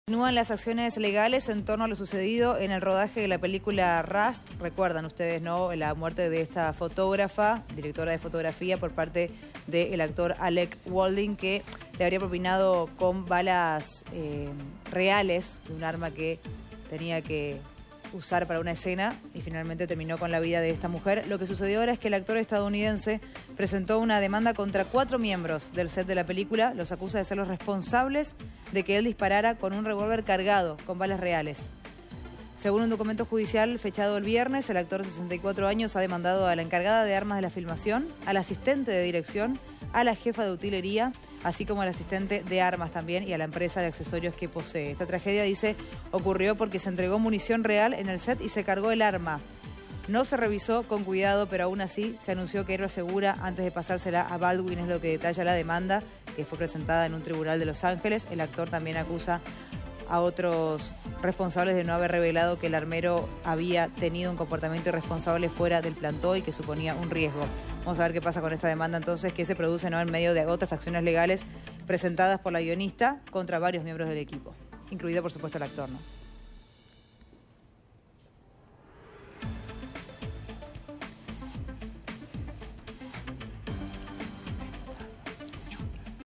EEUU: Alec Baldwin demanda a cuatro personas por la tragedia en el set de la película Rust
👉🏻El actor estadounidense Alec Baldwin presentó una demanda contra cuatro miembros del set de la película "Rust". Los acusa de ser los responsables de que él disparará un revólver cargado con balas reales, por el cual murió la directora de fotografía Halyna Hutchins en 2021, una tragedia que conmocionó a Hollywood.